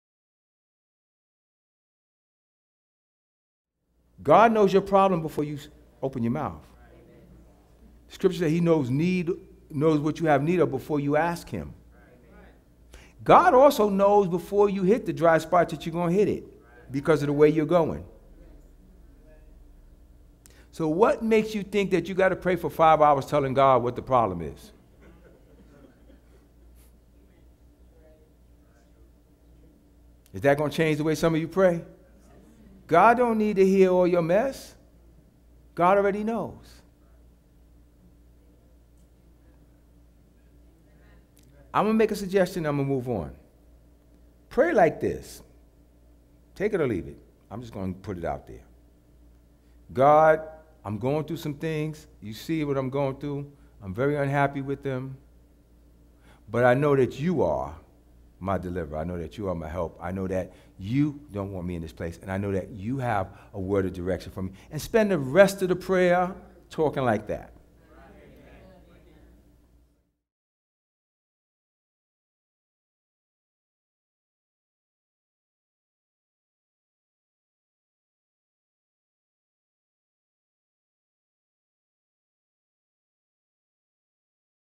Clip: "How To Pray" from the message "What Is Your Why? Pt 4"
Scriptures mentioned in video
Proverbs 3:7-9

Promise Land Church Outreach. * Joshua and Caleb Ministries, Inc. * Speaker: Pastor Di'Mon Brown* Place: 1688 Webster Ave. Bronx NY, 10457 *

Category
Film & Animation
License